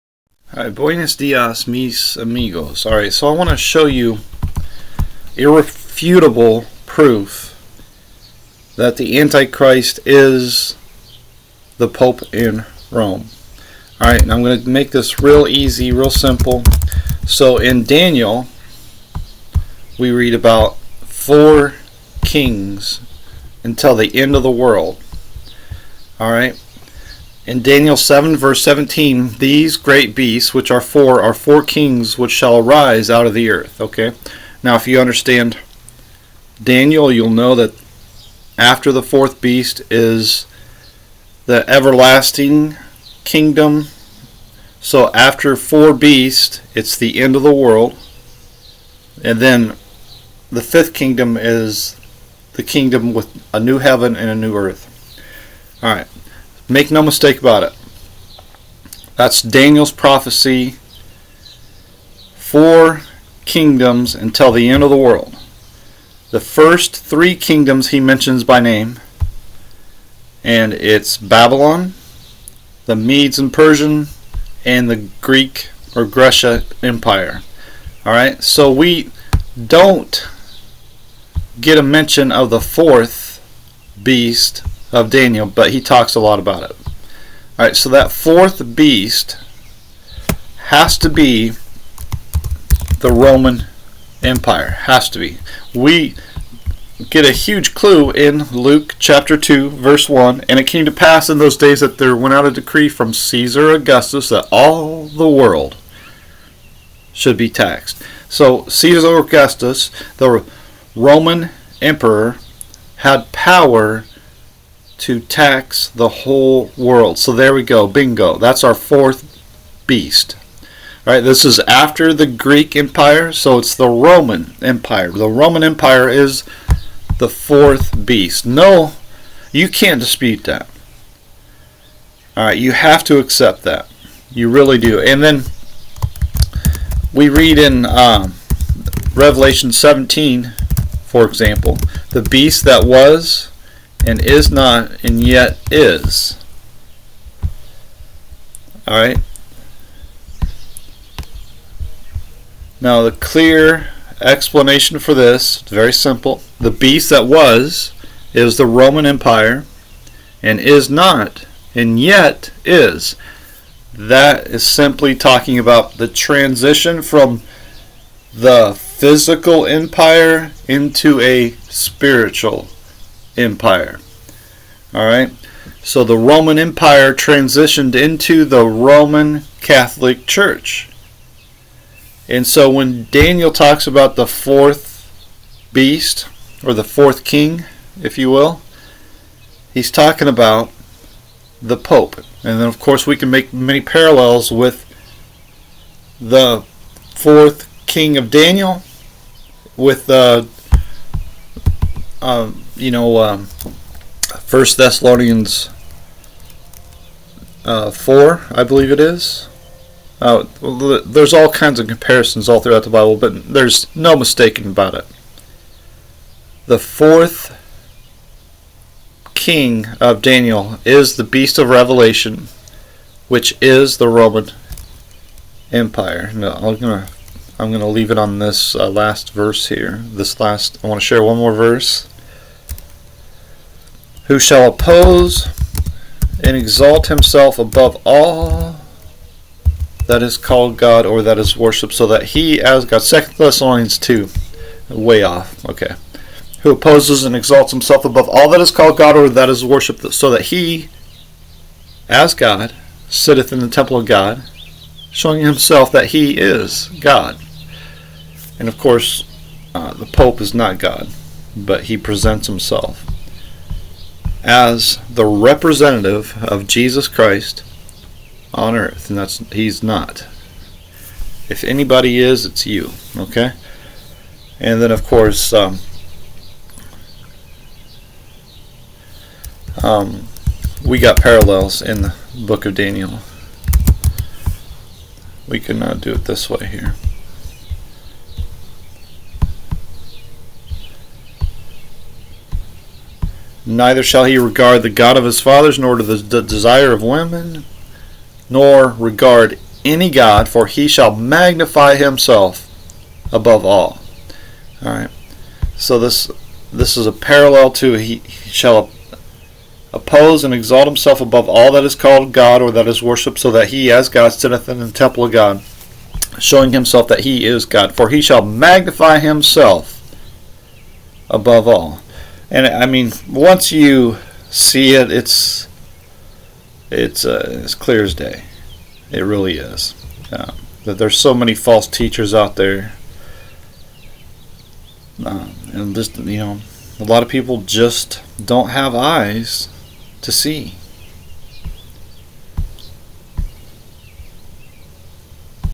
Irrefutable Proof the Pope is the Antichrist
Daniel 11:37
Neither shall he regard the God of his fathers, nor the desire of women, nor regard any god: for he shall magnify himself above all.

no desire for women... ever hear of a Mrs. Pope ?